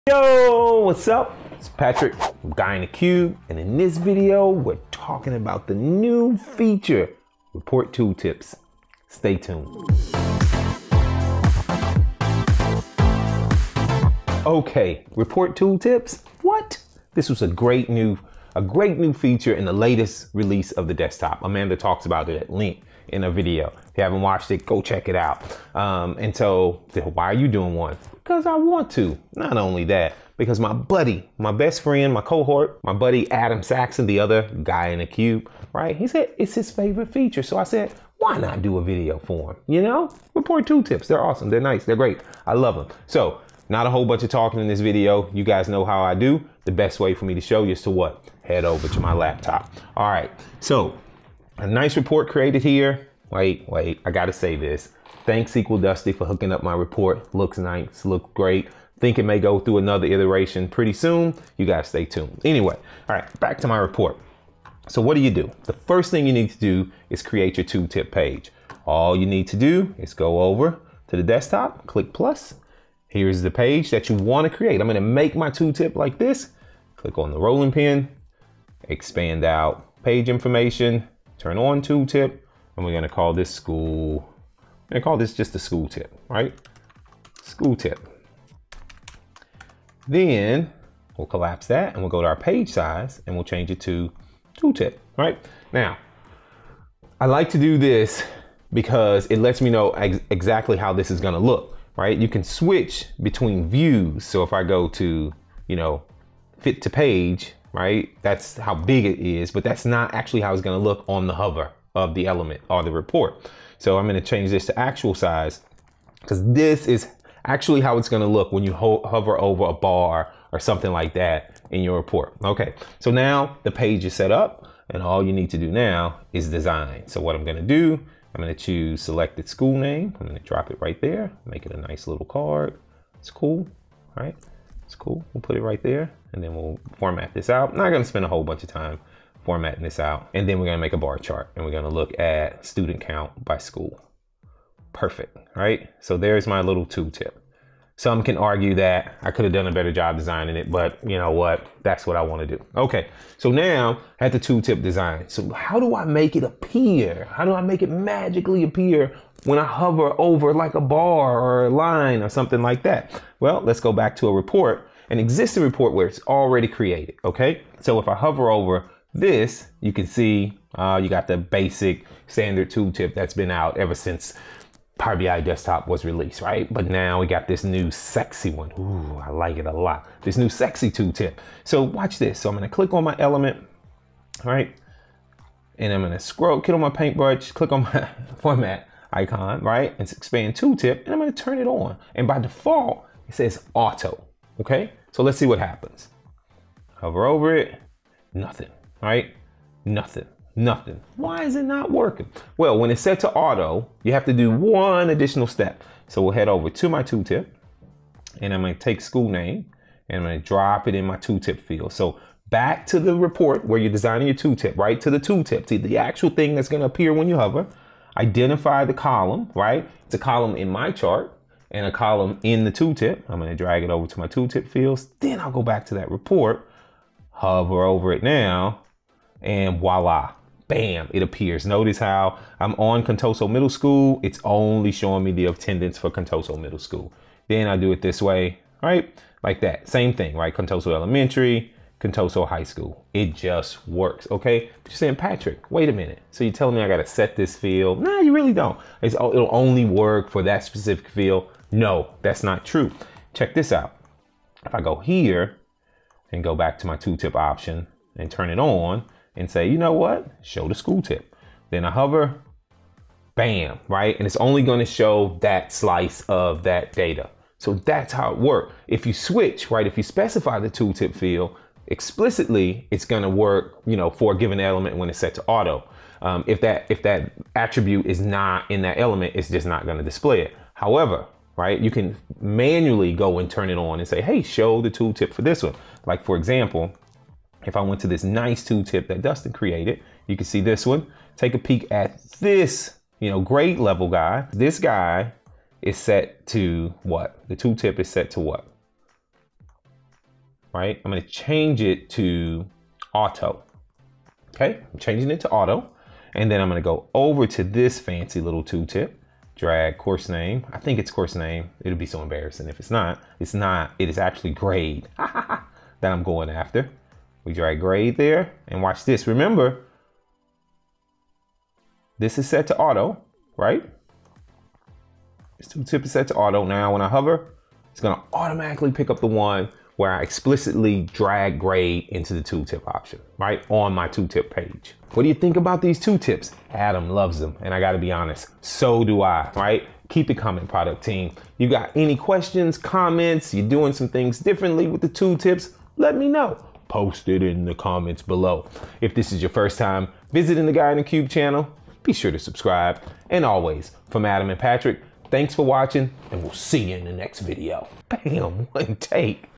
Power BI report page tooltip - How to create one in Power BI Desktop
Patrick walks through what a Power BI report page tooltip is and how to create one in Power BI Desktop. This is a feature that was added to Power BI Desktop in the March 2018 release.